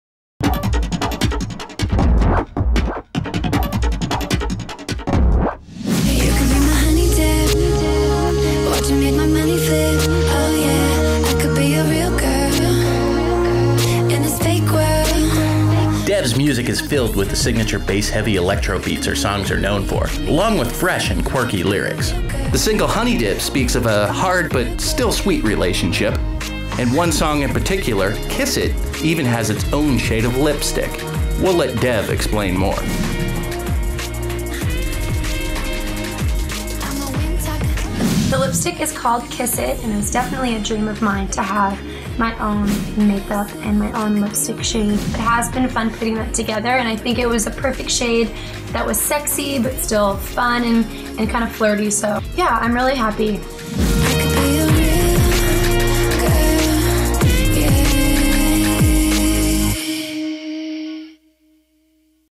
Listen Up - DEV (Lipstick)
DEV tells us what it's like to have her very own shade of lipstick.